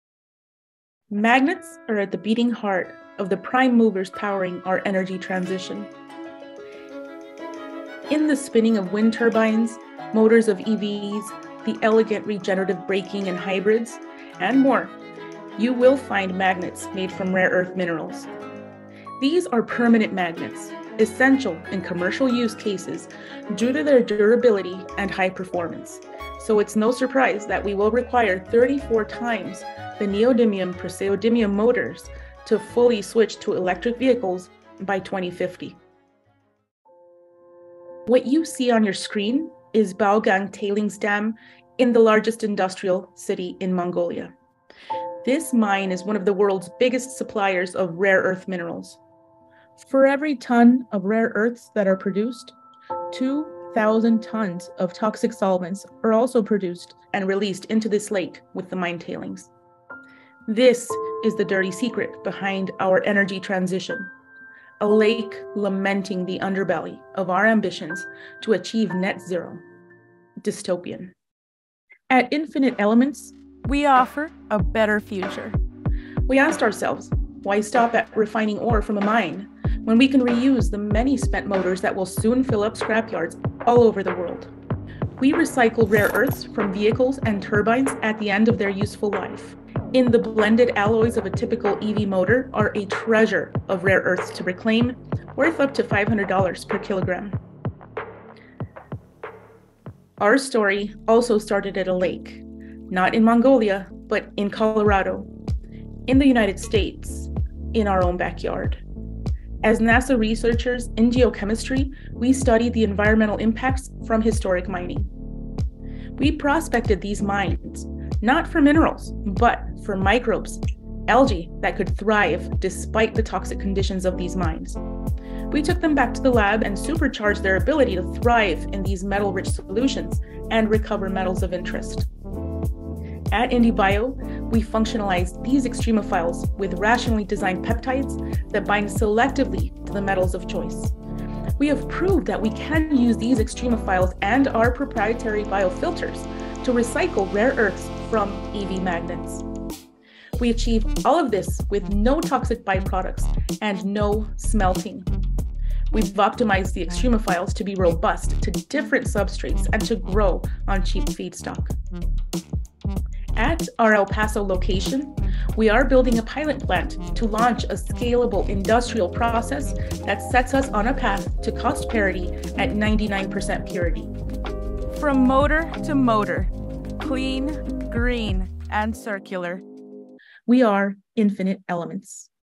Infinite Elements - IndieBio SF Demo Day 13
Infinite Elements harnesses extremophiles to extract, concentrate, and purify rare earth metals found in magnets. Their process enables sustainable recycling of the key inputs to the industries powering the energy transition. No toxic effluents, low energy, on-shore.